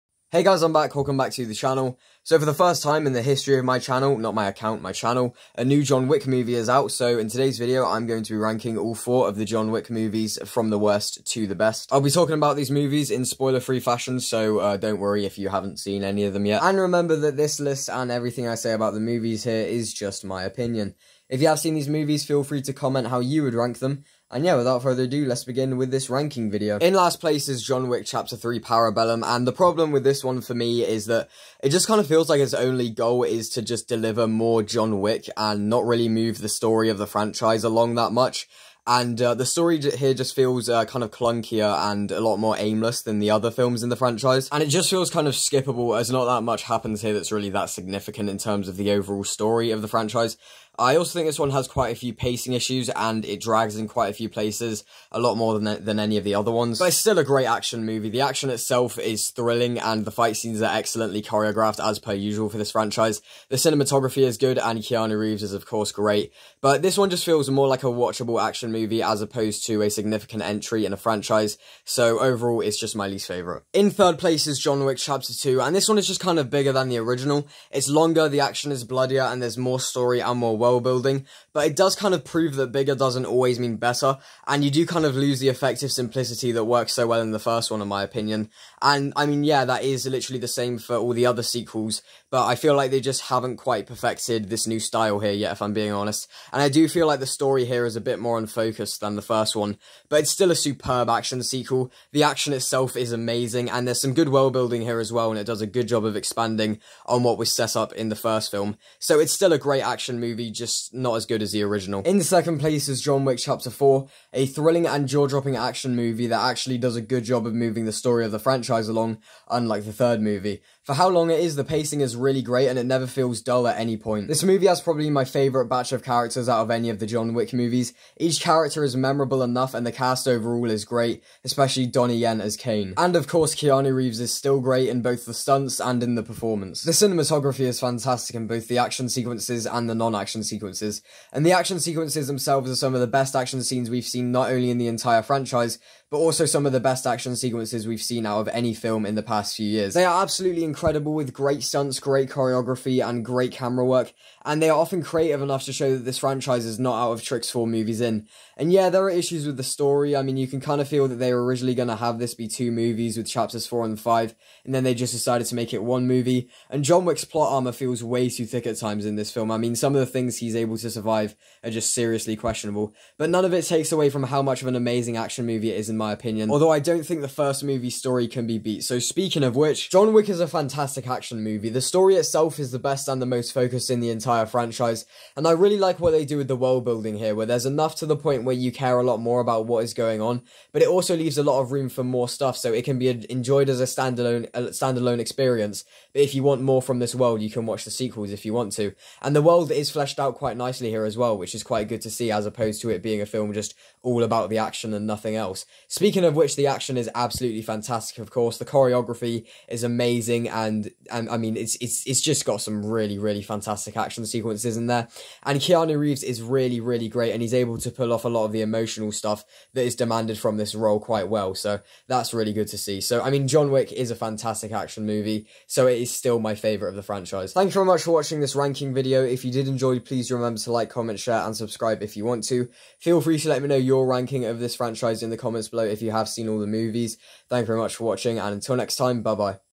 All 4 John Wick Movies Ranked Worst To Best!
Remember that this list is JUST MY OPINION

MOVIES TO RANK:
John Wick (2014)
John Wick: Chapter 2 (2017)
John Wick: Chapter 3 - Parabellum (2019)
John Wick: Chapter 4 (2023)